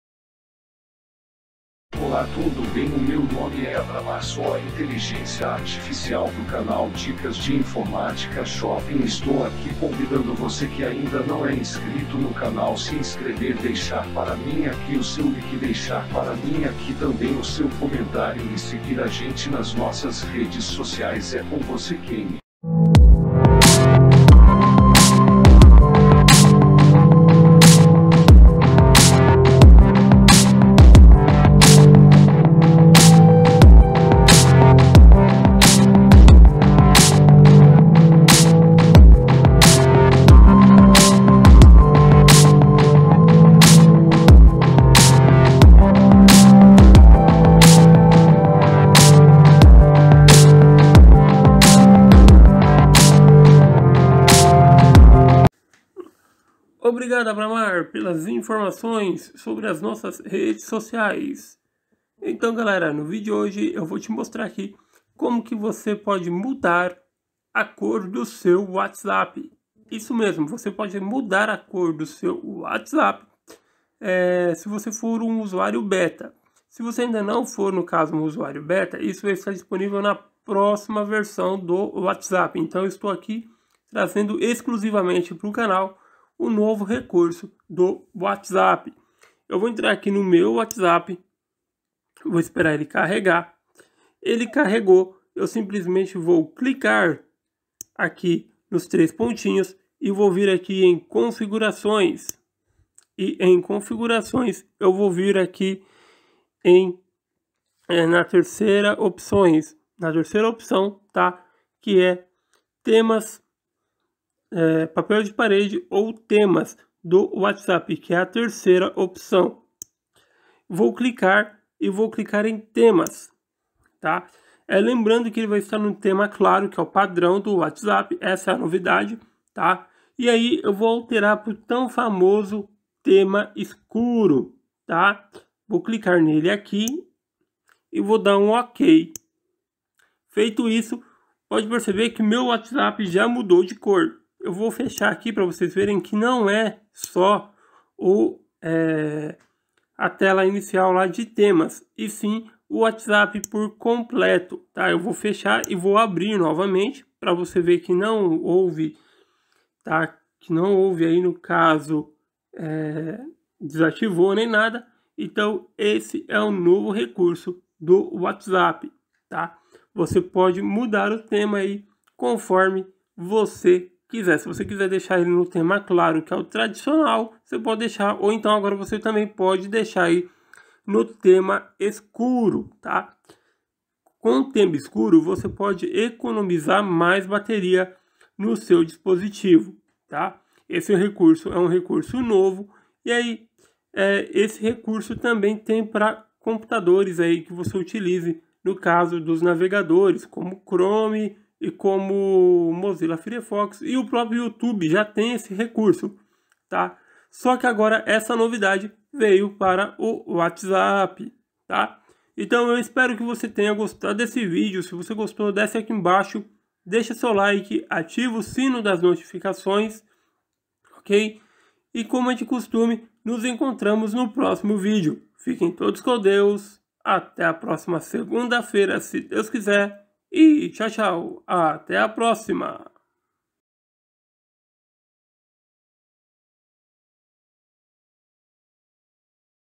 Conheça o novo recurso do WhatsApp nas próximas atualizações
No vídeo de hoje eu vou te mostrar a nova função do WhatsApp que é o tema escuro se você gostar desse vídeo já deixa aqui seu like ative o sininho das notificações e deixa para mim aqui o seu comentário o que você achou desse novo tema lembrando que esse tema ainda está em fase Beta então se você é um usuário Beta você já pode ativar conforme eu vou te mostrando no vídeo agora se você não é usuário Beta essa função só vai estar disponível nas próximas atualizações do WhatsApp

social